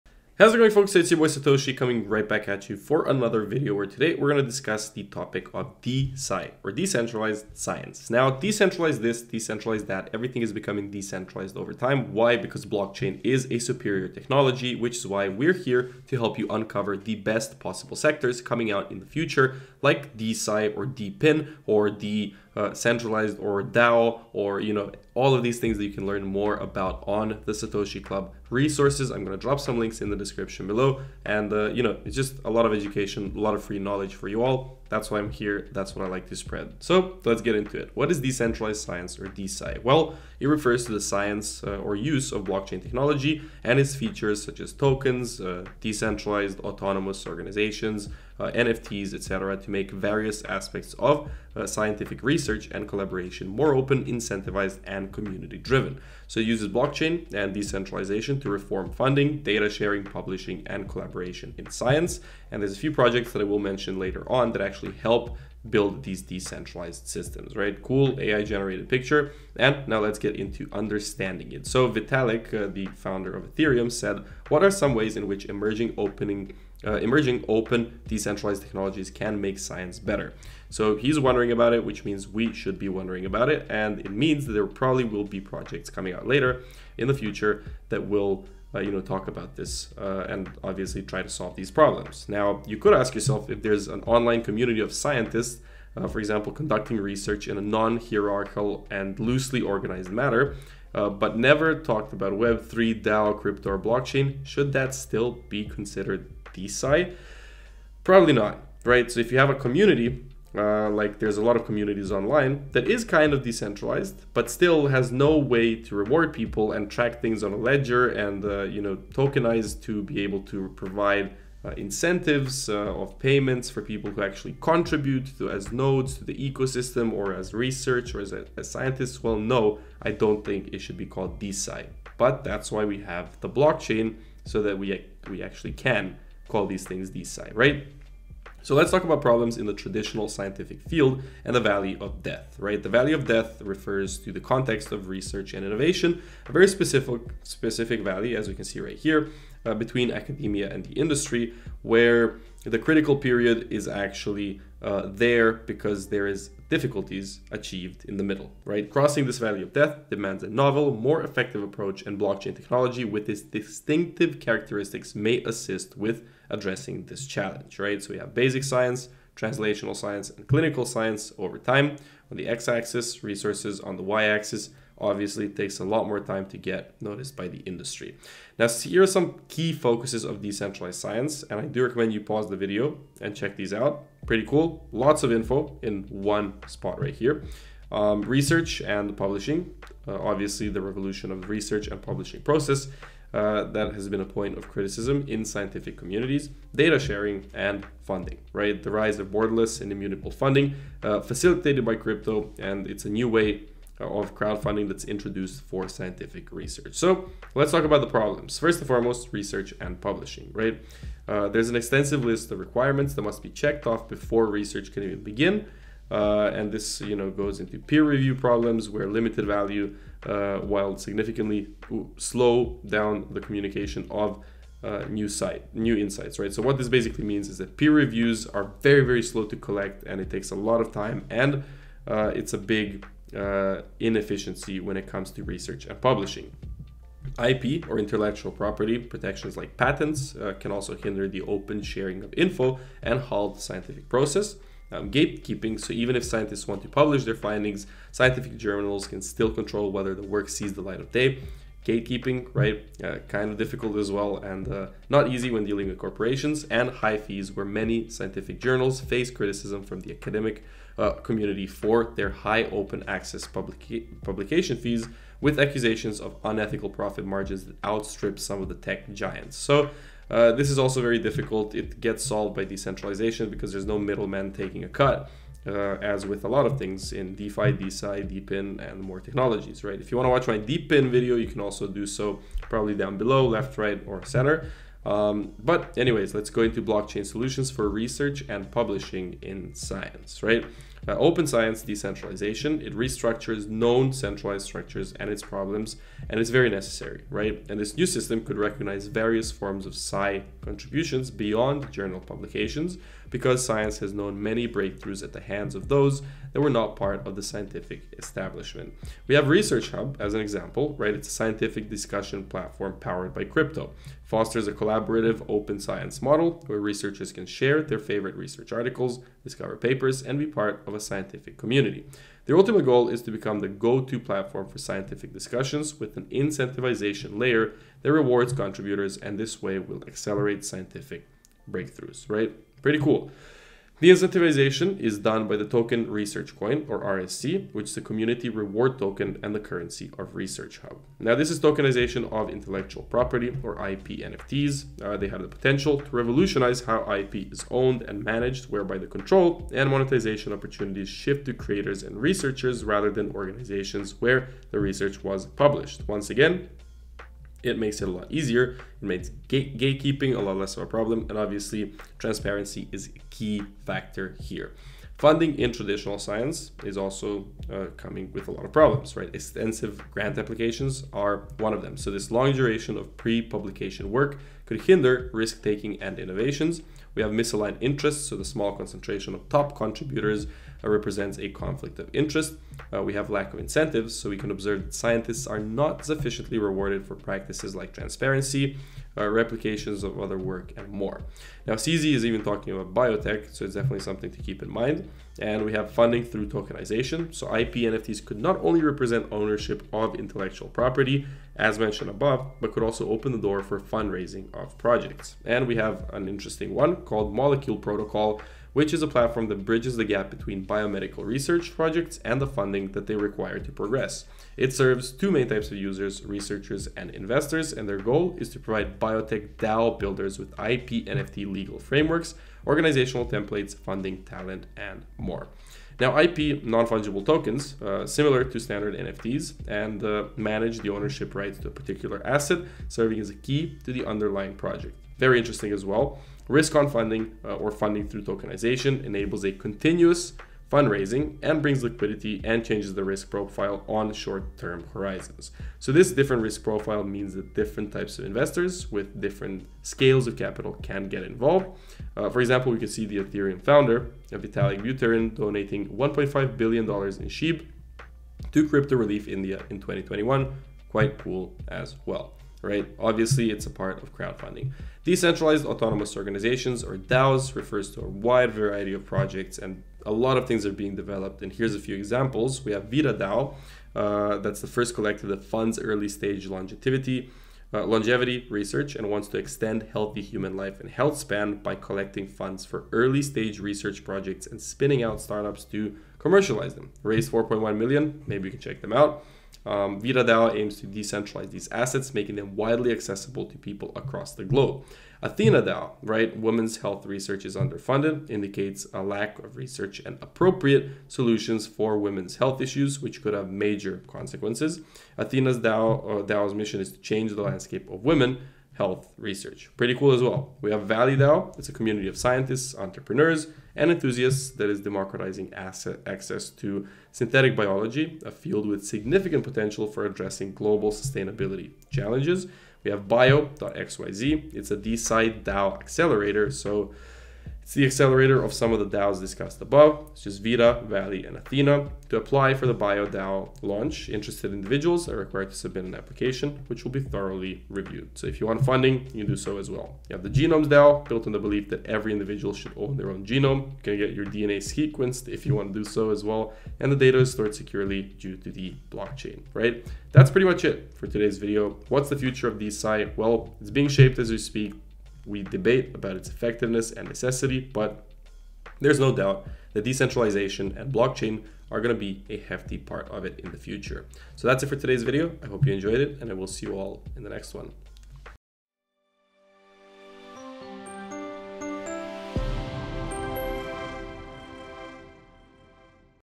What is Decentralized Science (DeSci) and How Does It Work on Blockchain?
🎯📊Decentralized science refers to the use of blockchain technology, and its features such as tokens, NFTs, and decentralized autonomous organizations (DAOs), to make various aspects of scientific research and collaboration more open, incentivized, and community-driven. Learn more in this video!💡

📌Timestamps:
0:00 Why DeSci
1:13 Vitalik Buterin's Thoughts on DeSci
3:27 Traditional Research Problems
4:47 Blockchain Solutions for Science Research Issues
6:40 DeSci Projects: Molecule Protocol, VitaDAO, Athena DAO, Bio.xyz, Genomes DAO
9:08 - DeSci Conclusions